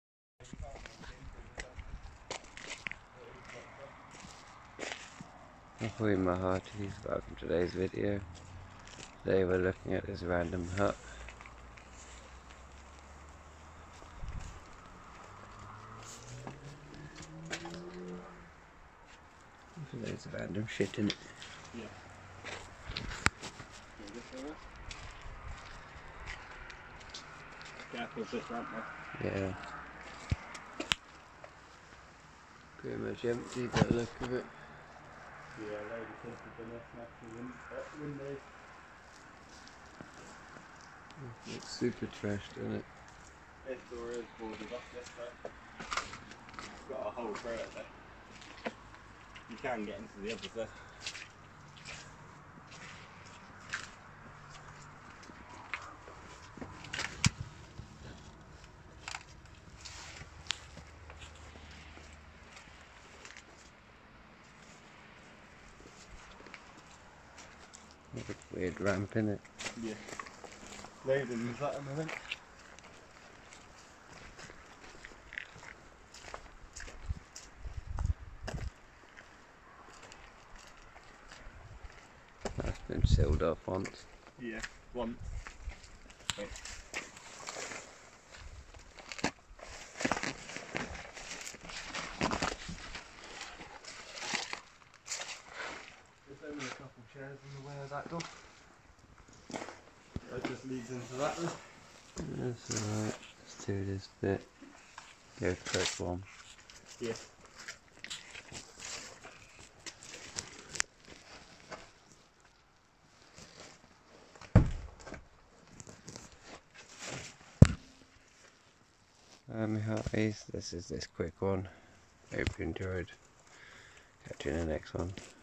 random abandoned hut
Quick explore of a abandoned hut hope you injoy